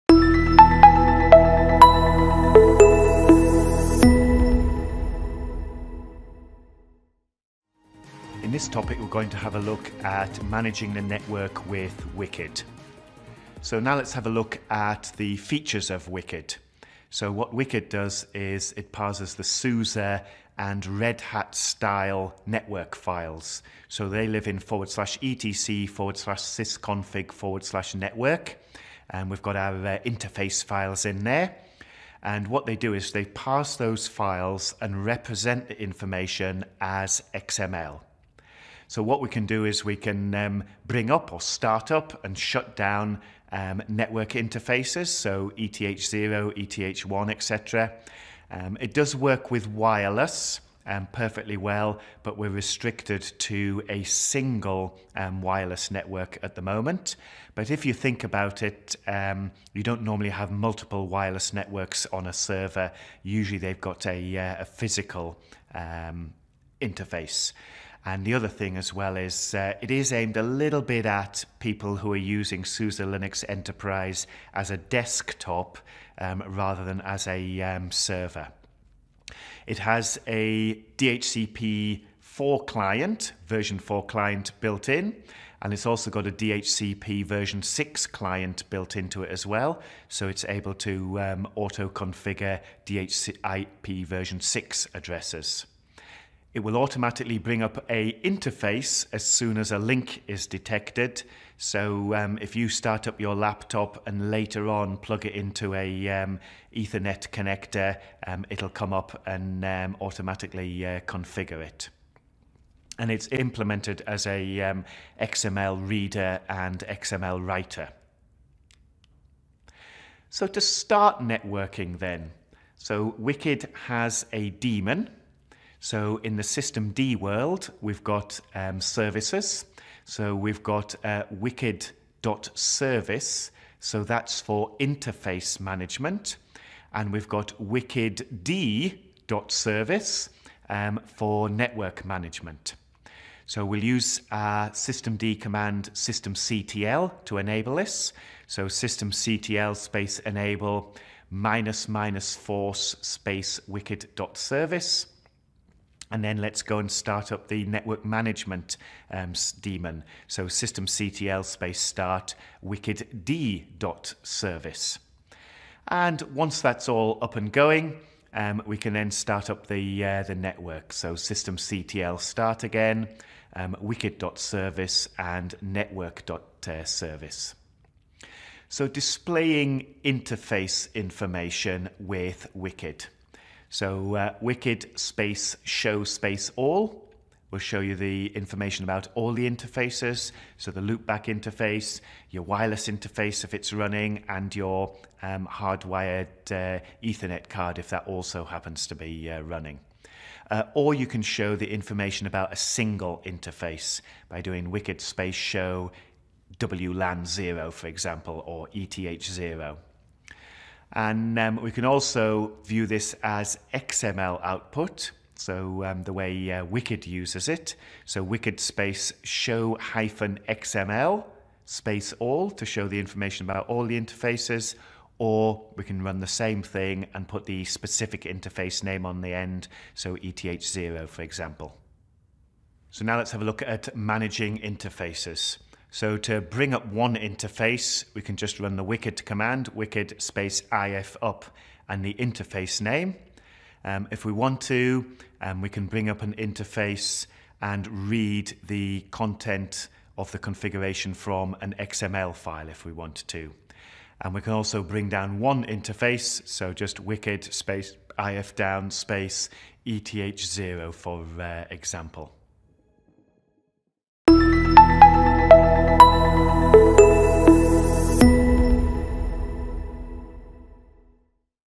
Sample SUSE eLearning Course: Understand Network Management with Wicked
In this sample course, you’ll get a taste of what the eLearning platform is and how it works. This sample course “Understanding Network Management  with Wicked” is part of our SUSE Linux Enterprise Server 201 Administration Learning Path.  The complete learning path is designed for those who are seeking a fundamental understanding of the SUSE Linux Enterprise Server (SLES) operating system.